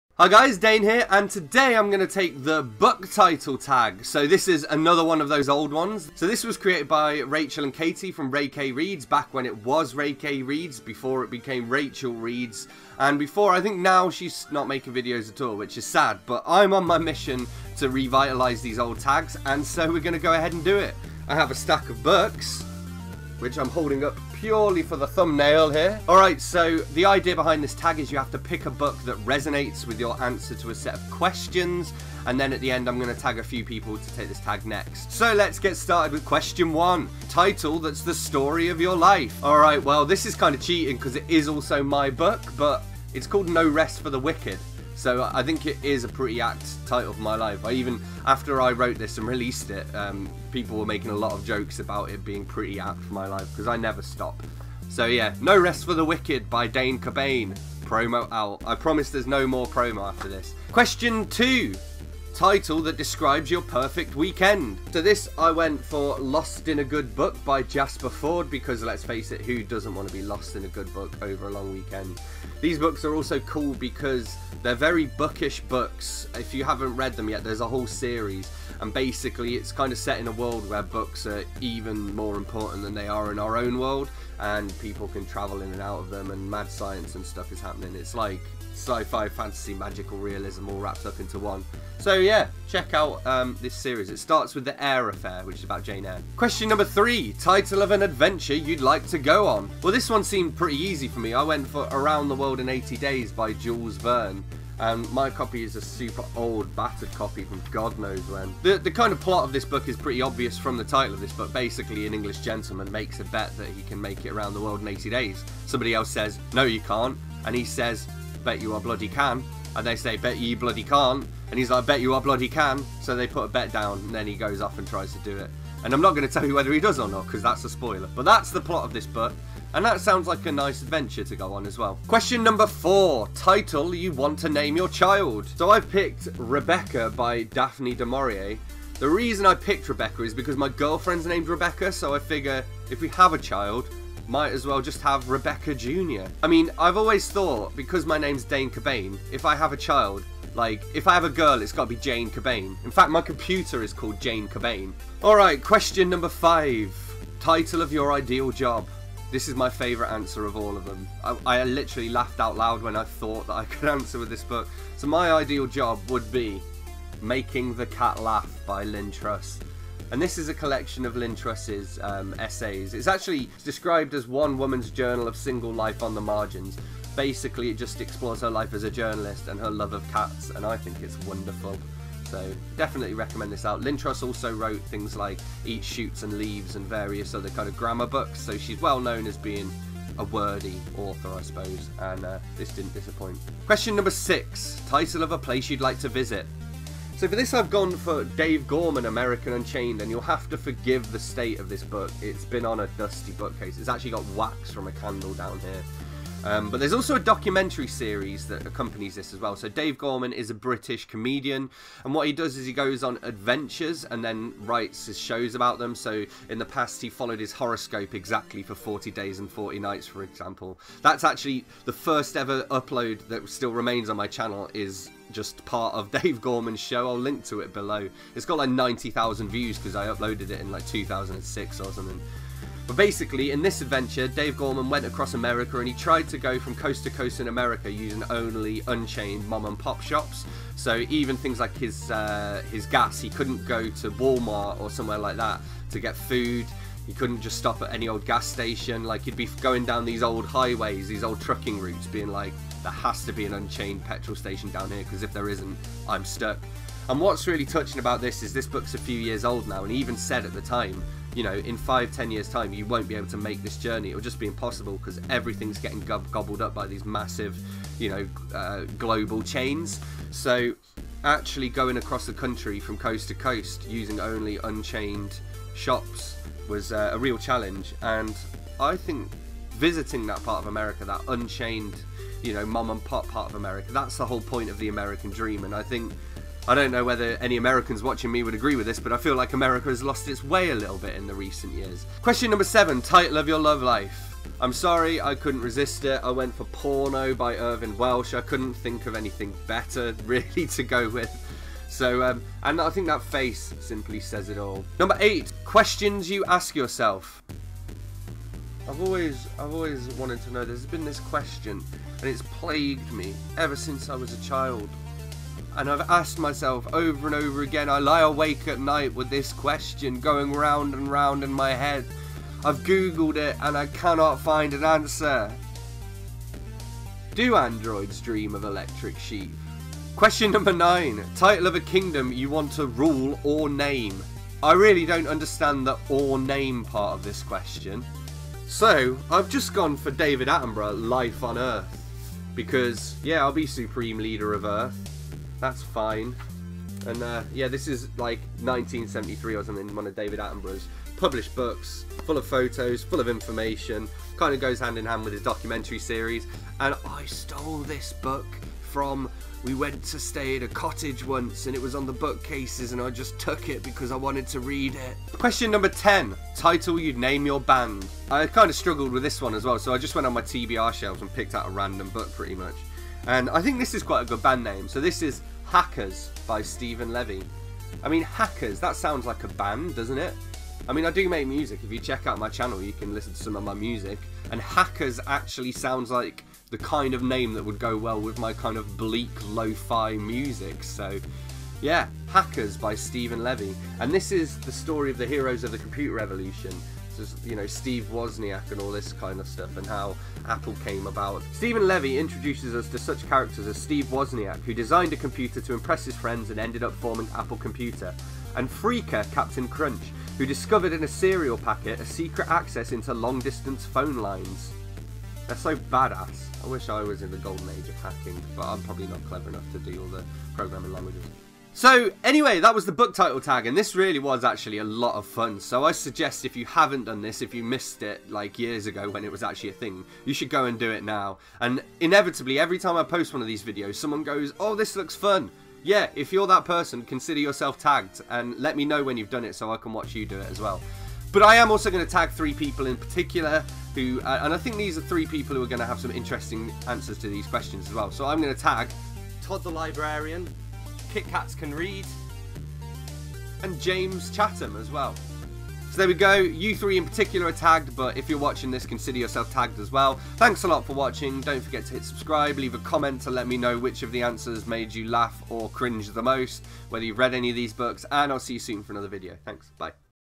The Book Title Tag! [BOOK TAG] | Tag Tuesday
It's tag time, because these things are fun.This was created by Rachel and Katie from RayKay / Rachel Reads

Pick a book title that resonates with these questions:

1. Title that's the story of your life
2. Title that describes your perfect weekend
3. Title of an adventure you'd like to go on
4. Title you want to name your child
5. Title of your ideal job
6. Title of a place you'd like to visit
7. Title of your love life
8. Questions you ask yourself
9. Title of a kingdom you want to rule OR name
10. Title you'd name your band.